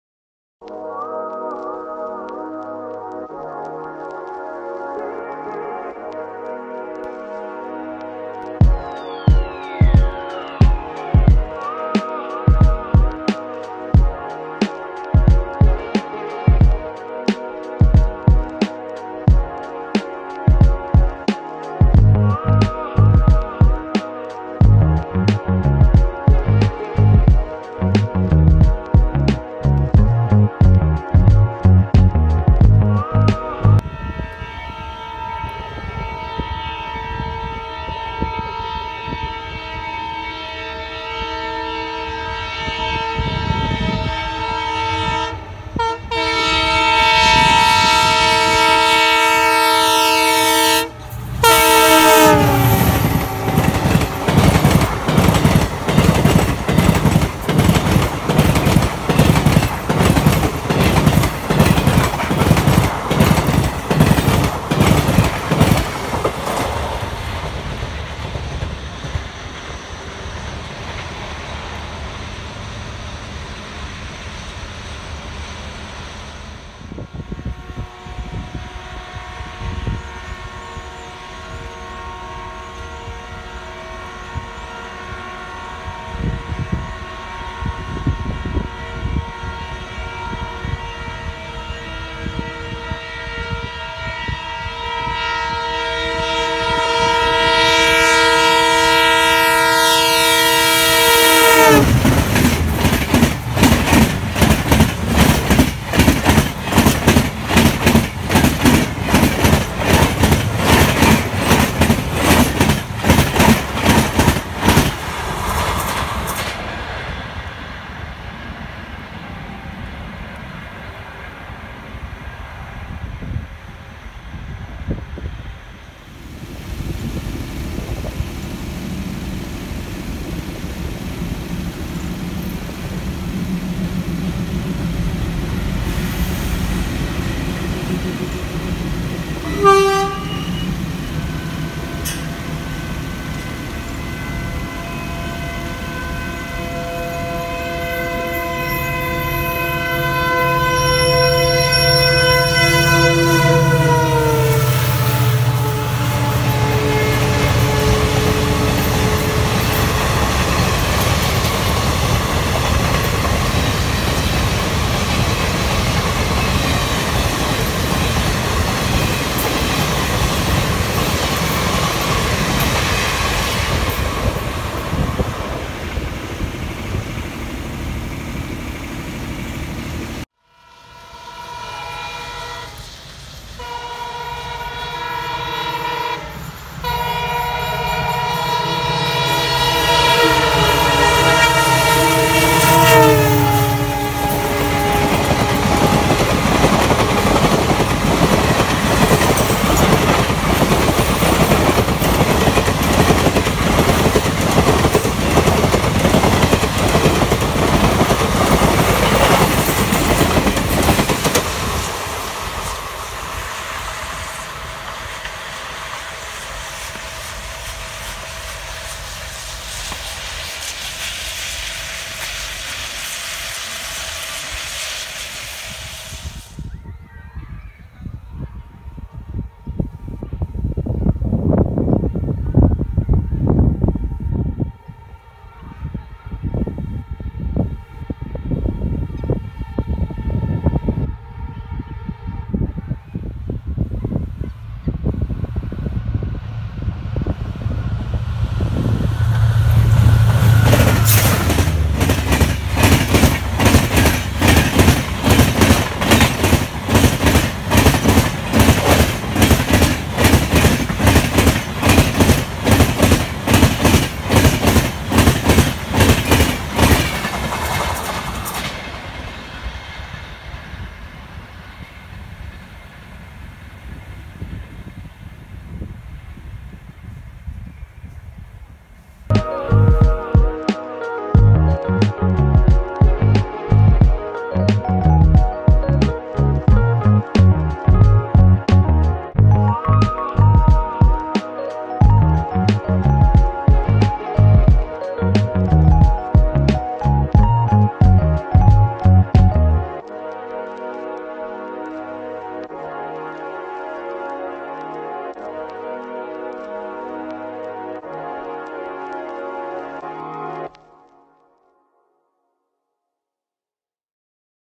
धुल उड़ाती तेज़ सुपरफास्ट ट्रेनें । Heavy Dust Storm Created By Superfast Trains | Indian Railways
Superfast Trains Creating Heavy Dust Storms At There Maximum Permissible Speed !

Trains In The Sequence Are :

1. 12195 - Agra Fort Ajmer Intercity Express.
2. 12015 - Ajmer Shatabdi Express.
3. 15014 - Ranikhet Express.
4. 12181 - Dayodaya Express.
5. 22631 - Anuvrat AC Superfast.

Dhanakya(NWR) - December 2018 !